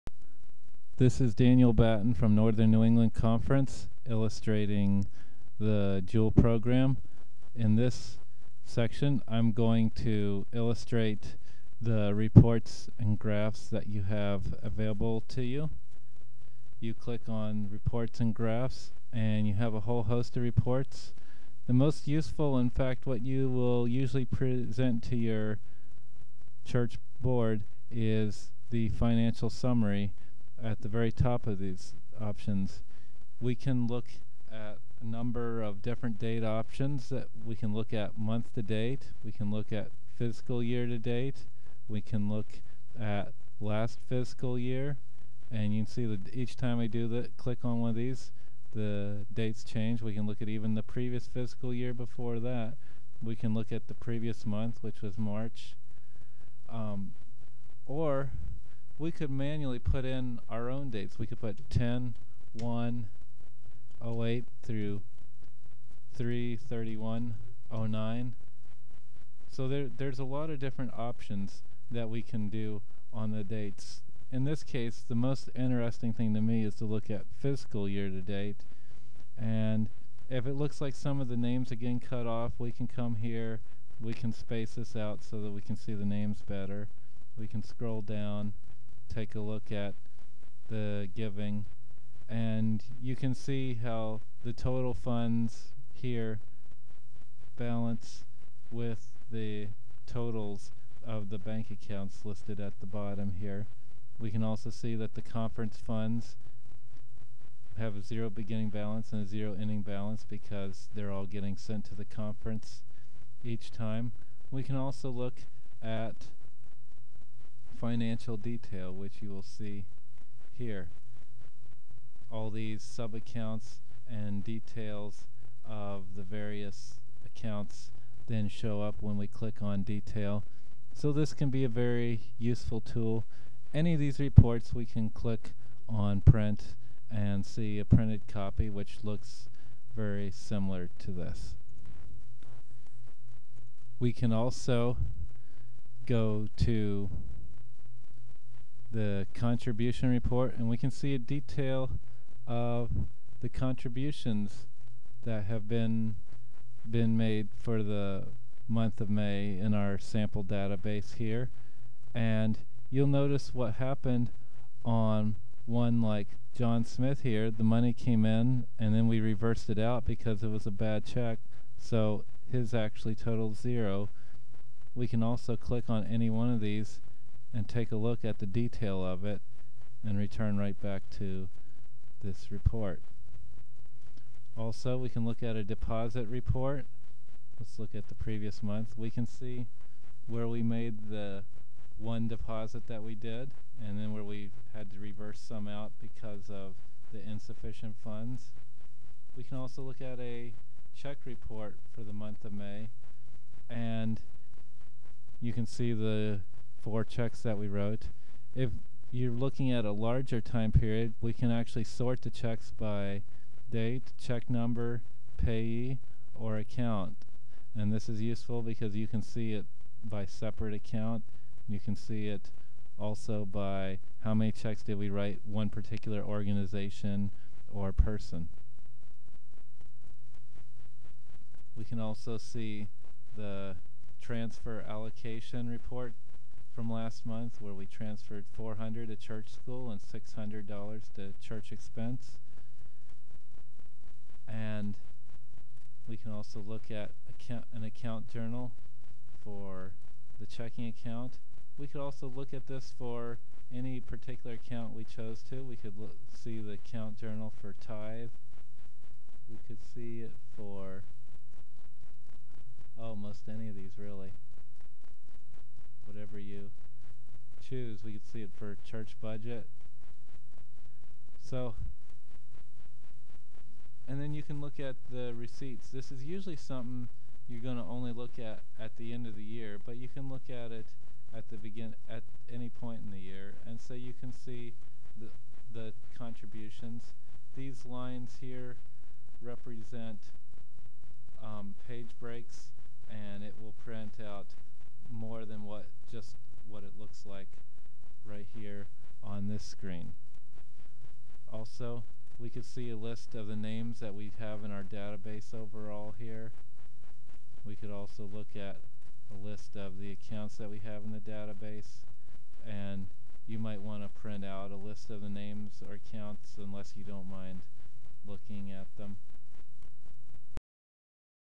Jewel Reports
How to access and use reports and graphs in the Jewel SDA Church Accounting program.